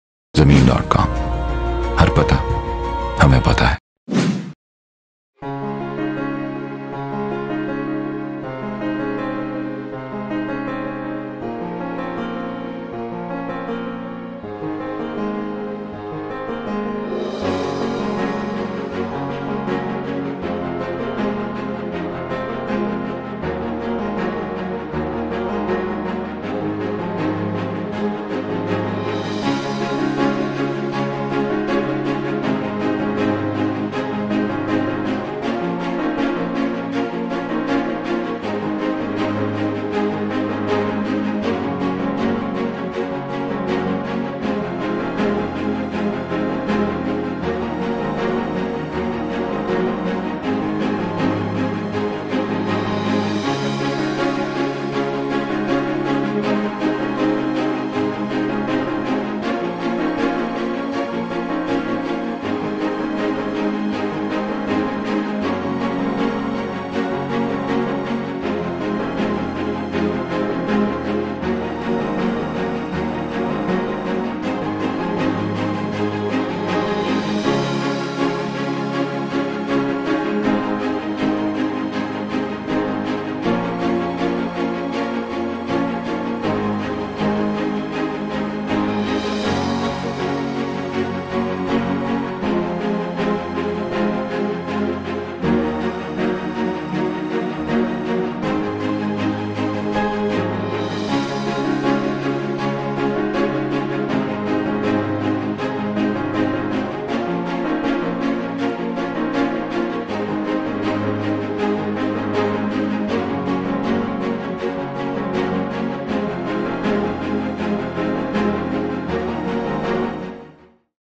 2.8 MARLA APARTMENT FOR SALE IN ZARKON HEIGHTS G-15 ISLAMABAD
Zarkon Hieght CDA APPROVED
- 15% Down payment | 4 Years equal installment plan
- 1 Bed - 4 Bed Duplex Luxury Gated Apartments
- 2 Towers Handed over
- Mosque, School, Community Center
- 2 Floors Basement Parking
- Power Back-up | 24 Hrs Security | Wapda | SNGPL Approved and installed
- Main G. T Road Sector G-15 Islamabad
- 10 KMs away from the New International Islamabad Airport.
Water, Electricity and 24 hour security with CCTV. Electric geyser. 24/7 Electricity Back up.